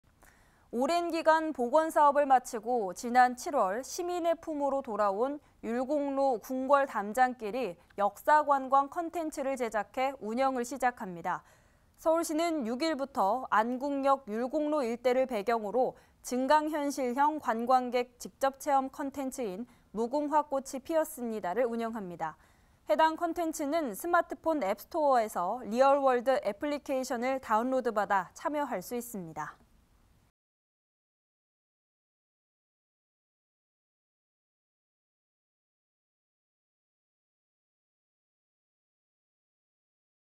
서울시, '율곡로 궁궐담장길' 관광 체험 콘텐츠 운영 _SK broadband 서울뉴스
[기사내용]
오랜 기간 복원 사업을 마치고,
지난 7월 시민 품으로 돌아온 '율곡로 궁궐담장길'이
역사 관광 콘텐츠를 제작해 운영을 시작합니다

서울시는 6일부터
안국역 율곡로 일대를 배경으로
증강현실형 관광객 직접 체험 콘텐츠인
'무궁화 꽃이 피었습니다'를 운영합니다.

해당 콘텐츠는
스마트폰 앱스토어에서
리얼월드 어플리케이션을 다운로드 받아
참여 가능합니다.

(영상편집ㅣ전현표 기자)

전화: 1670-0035